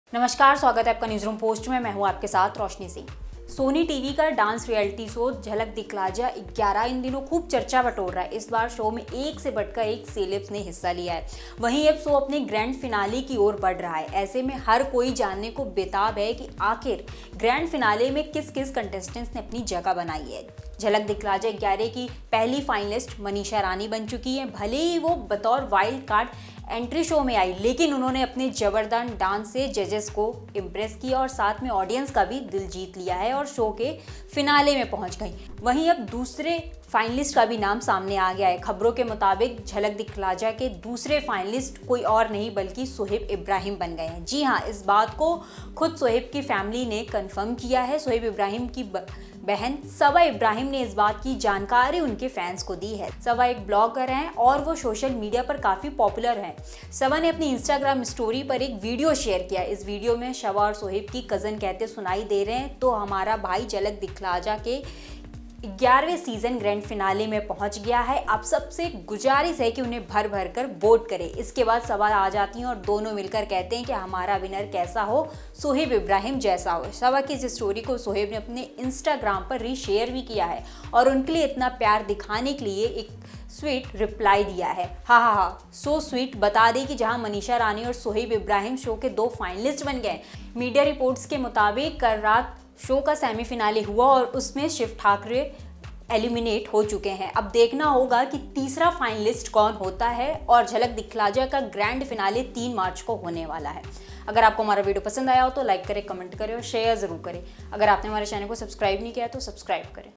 Jhalak Dikhhla 11 | मनीषा रानी के बाद ये कंटेस्टेंट बना दूसरा फाइनलिस्ट, फैंस को लगा तड़का झटका...
Jhalak Dikhhla 11 | Sony TV's dance reality show Jhalak Dikhhla Jaa 11 is creating a lot of buzz these days. This time many celebs have participated in the show. At the same time, now the show is moving towards its grand finale. In such a situation, everyone is eager to know which contestants have made their place in the grand finale. Manisha Rani has become the first finalist of Jhalak Dikhhla Jaa 11. Even though she came to the show as a wild card entry, she won the hearts of the judges as well as the audience with her tremendous dance and reached the finale of the show.